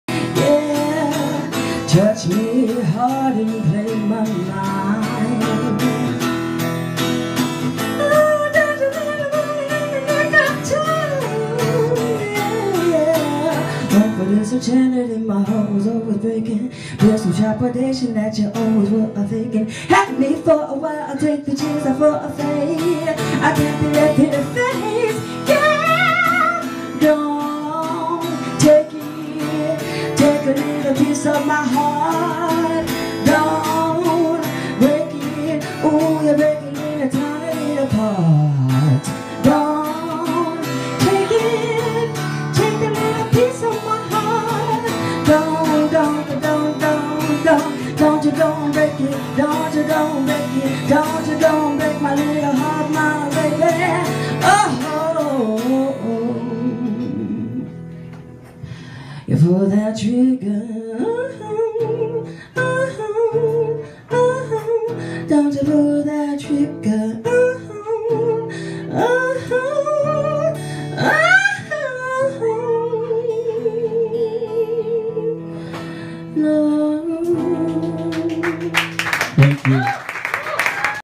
Marcus Harmon song Clip @OneMicNite™
Singer Marcos Harmon song clip at OneMicNite™, hosted by Marcos Luis. Event by 3SB Theatre Co